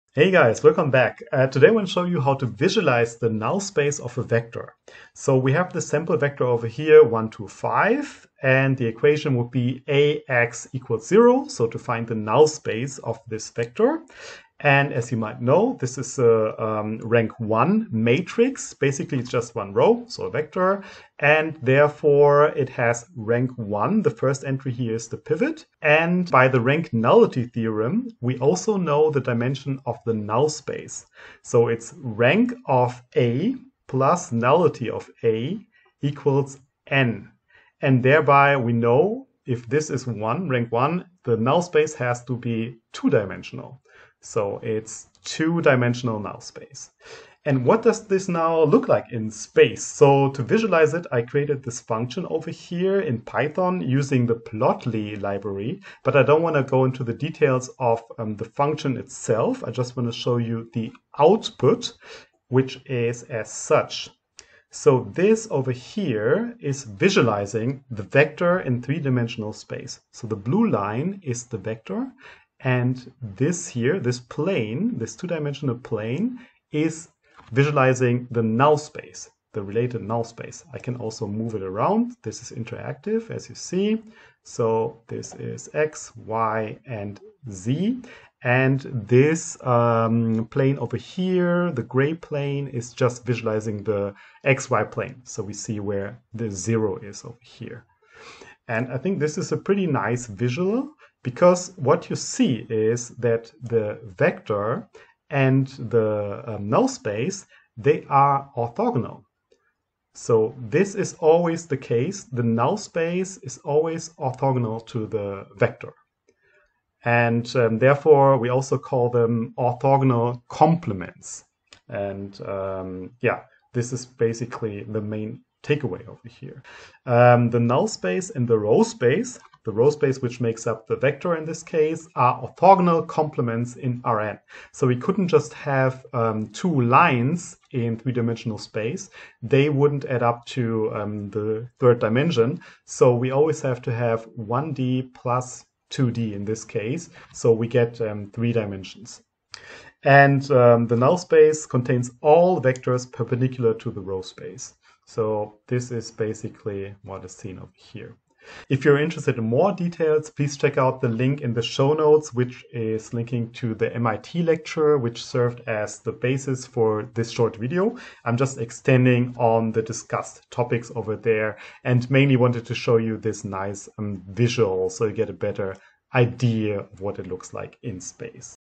Interactive Visualization of a Vector and Its Null Space
Explore a clear and interactive 3D visualization of a vector and its corresponding null space. This visual highlights the geometric relationship between a vector in 3D space and the plane orthogonal to it. Perfect for enhancing understanding of linear algebra concepts through visual aids.

If you want to learn more about the theoretical backgrounds check out MIT's lecture. The part after minute 28:00 discusses this example: